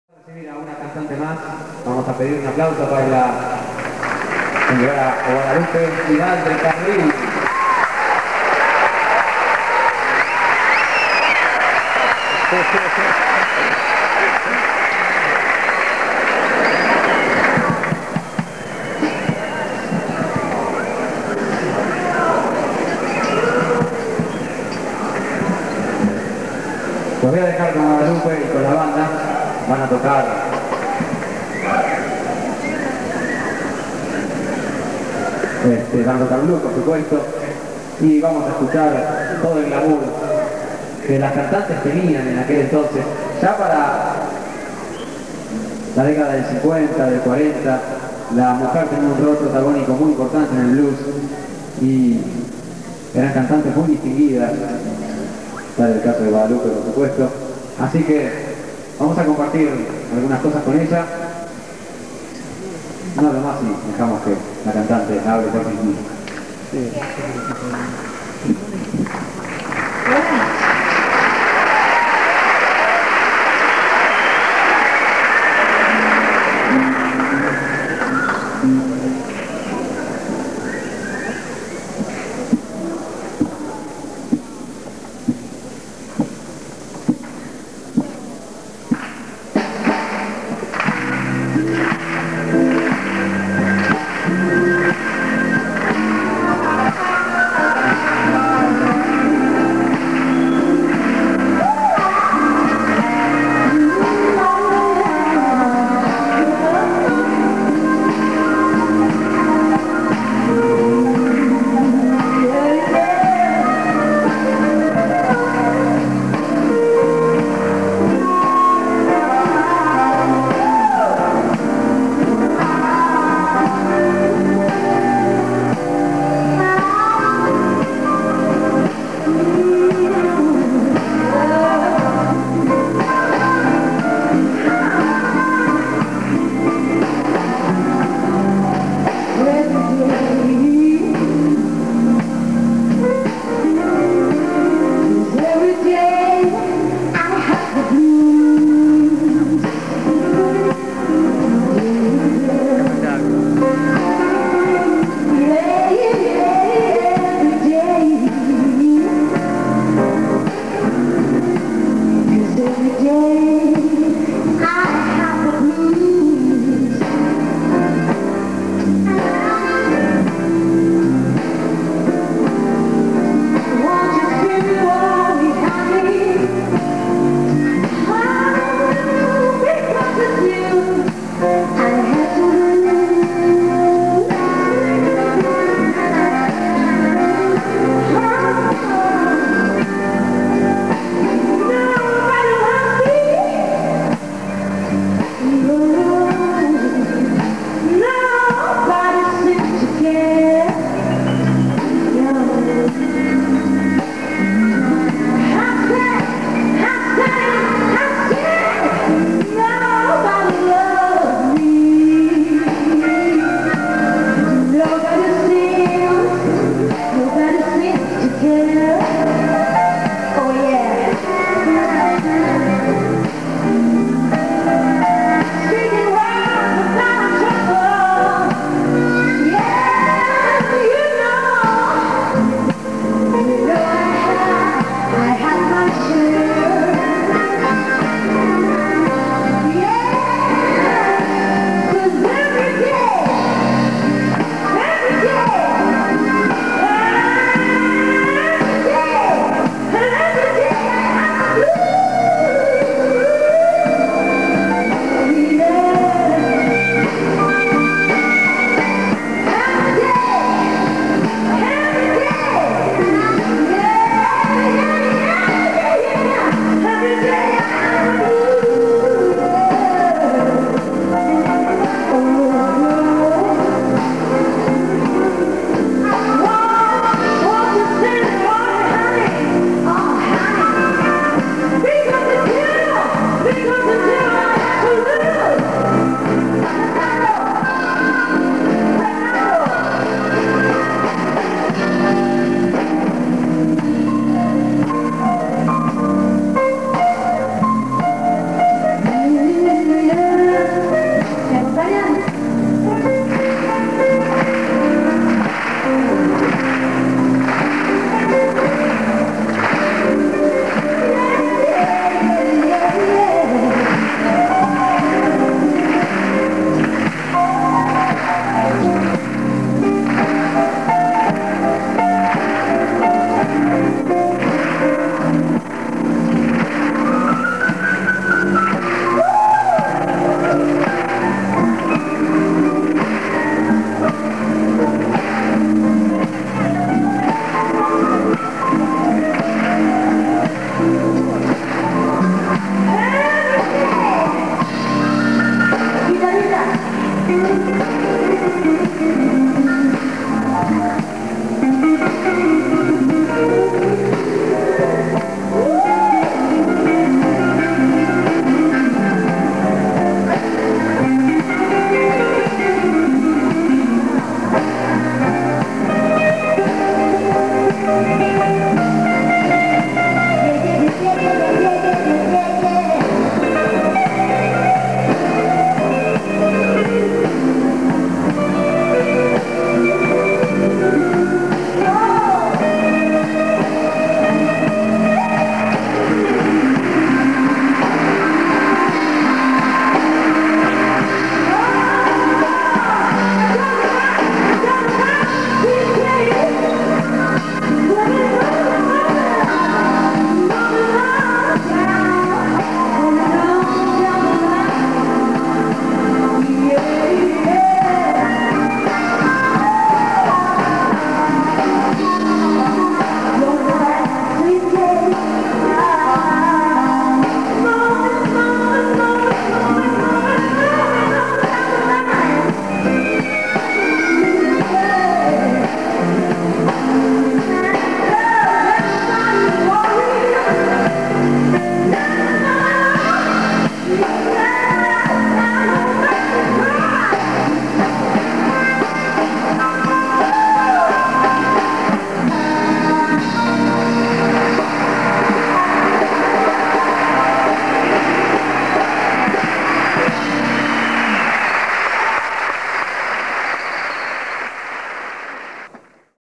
Daniel Raffo invitado para La Historia del Blues en el Teatro Colón de Buenos Aires -2002 -
Guadalupe Vidal del Carril (voz) Daniel Raffo (guitarra) Adrián Jimenez (armónica ) Gabriel Caviaglia (bateria ) Roberto Porzio (guitarra ) Fernando Tejero ( piano ) Mauro Diana (bajo)
La Historia del Blues en el Teatro Colon
Año 2002 🙏 Gracias por contribuir a mantener mi canal mediante GRACIAS o PAYPAL